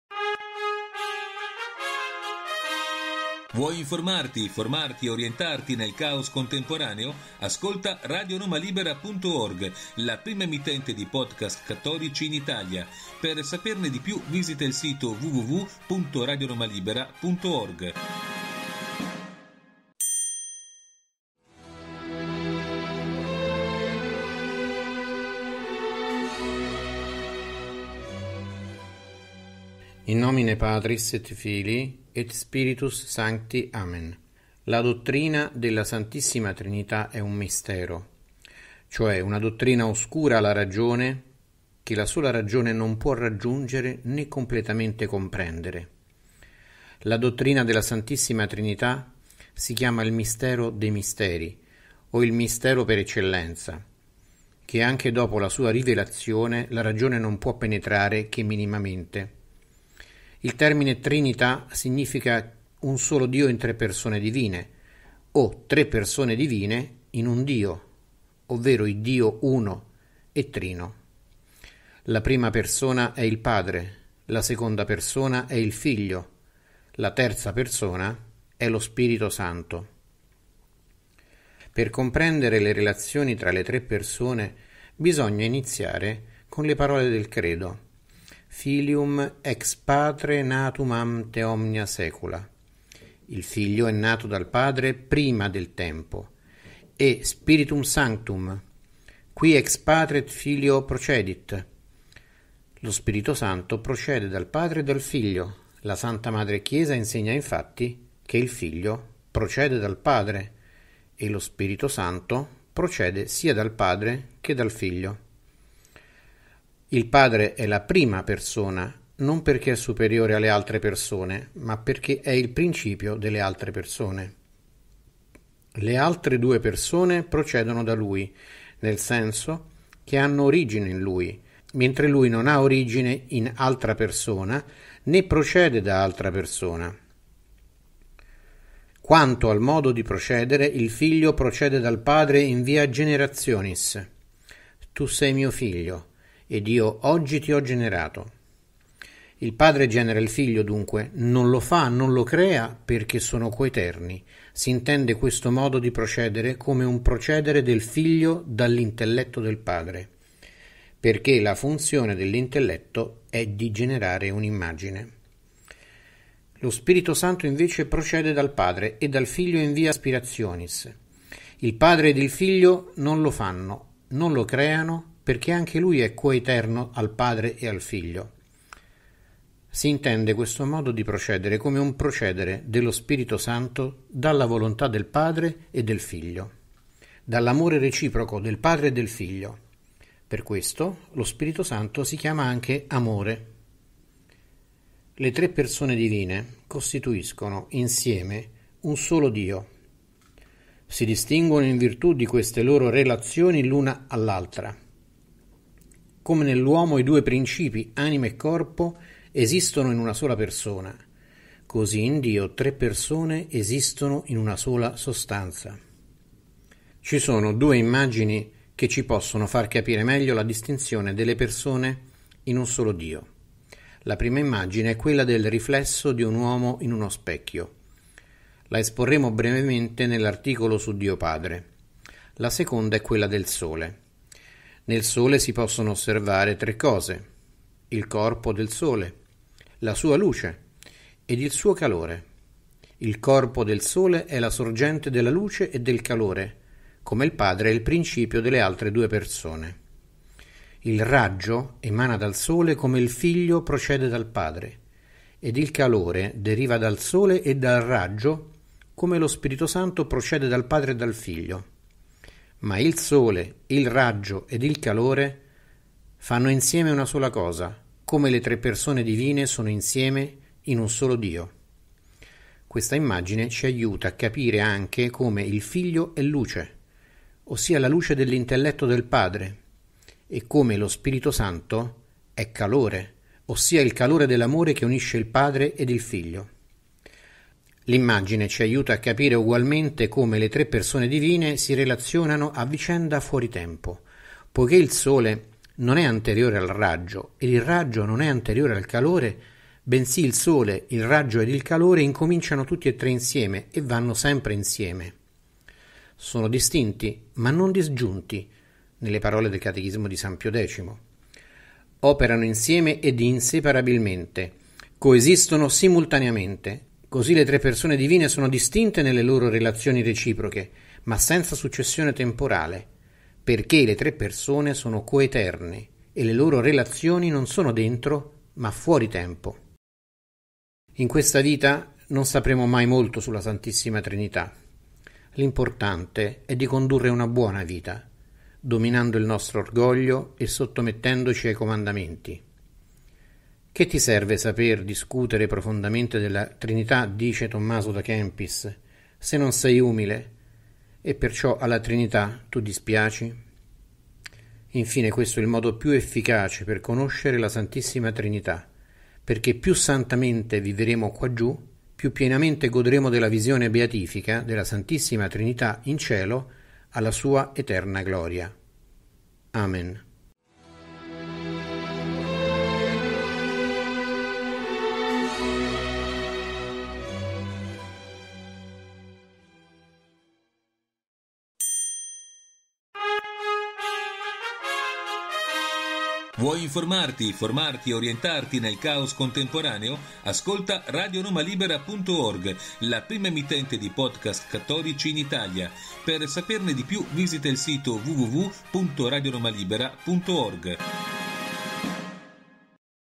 49 - Dio Trino
La dottrina della Santissima Trinità è un mistero, cioè una dottrina oscura alla ragione, che la sola ragione non può raggiungere né completamente comprendere.